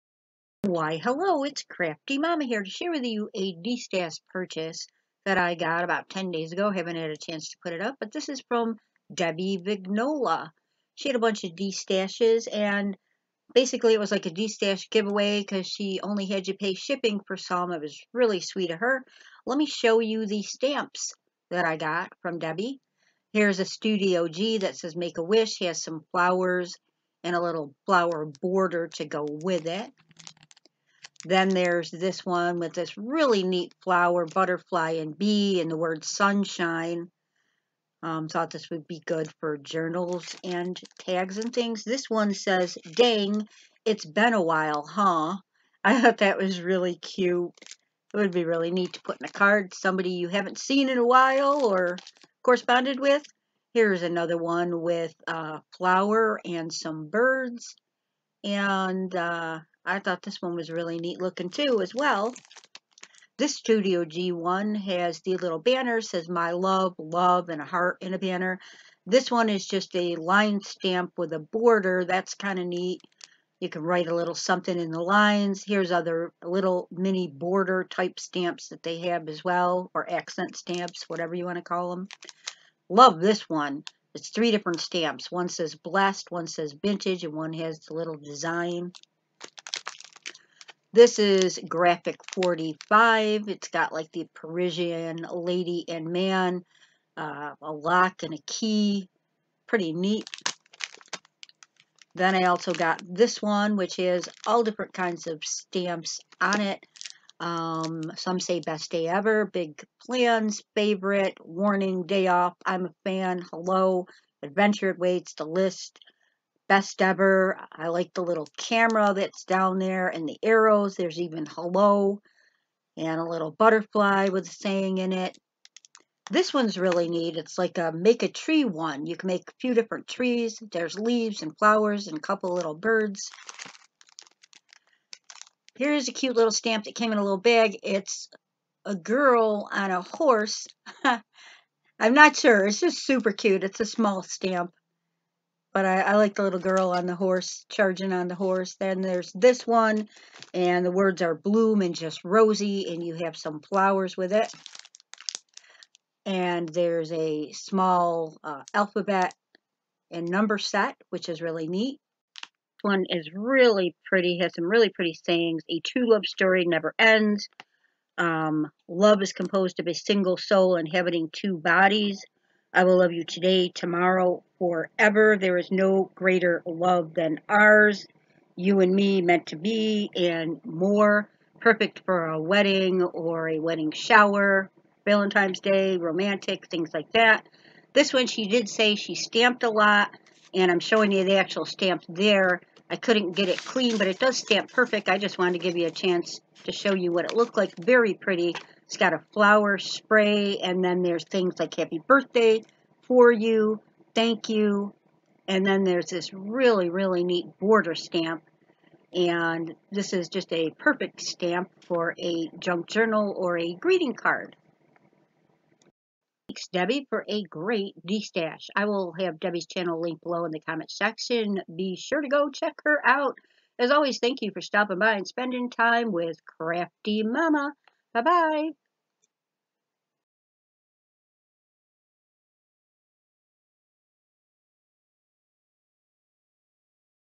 Destash Purchase From Debbie Vignola
I purchased a Stamp Lot Destash from Debbie Vignola, come see the array of stamps I got to create cards, journal pages & more with! Be sure to check out Debbie's channel